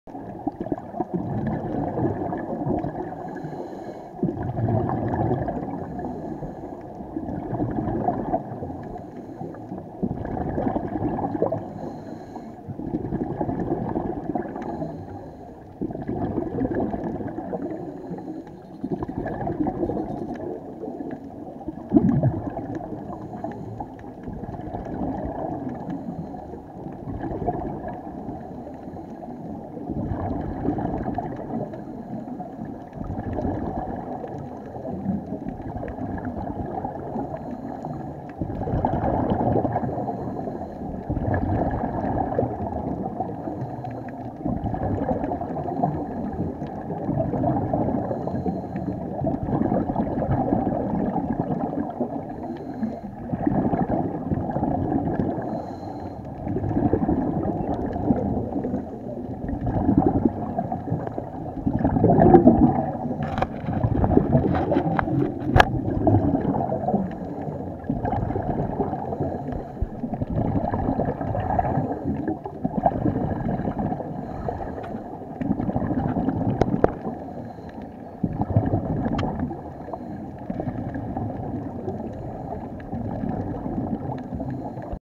My sea divg - January 4, 2019
Learning how to dive in open sea is a challenge .  Getting close to sea living resident is an exhilarating experience.  Awareness of Environment is also a good factor to understand. Protect  Mother Earth.